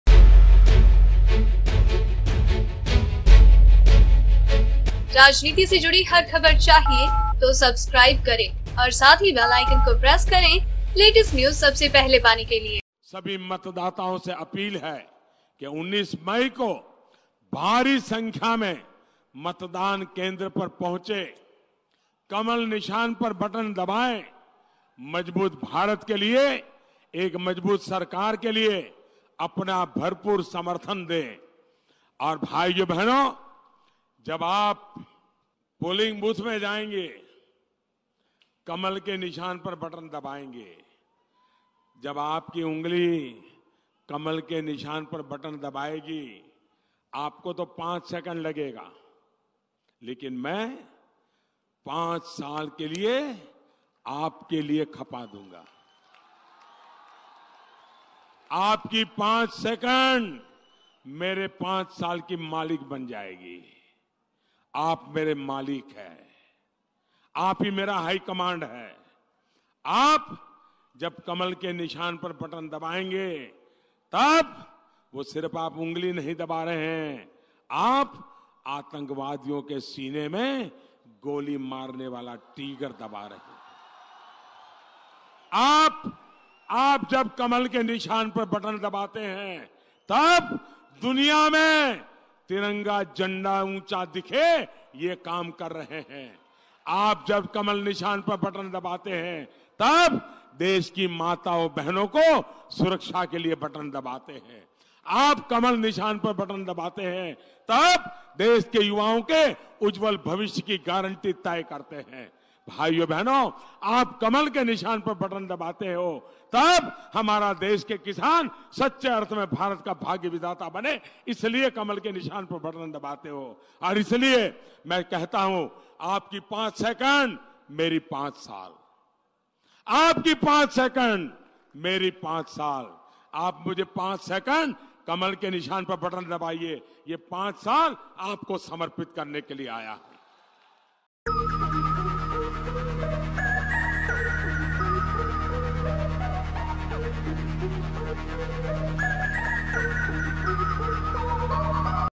आतंकवादियो को लेकर फिर बोले मोदी, कहा- कमल का बटन दबाओ अतंकवादियो के सीने में गोली चलाओ !
Narender Modi Against Congress | Narender Modi Speech Against On Congress | Narender Modi Latest Speech |

EDITOR ► Rakesh Sharma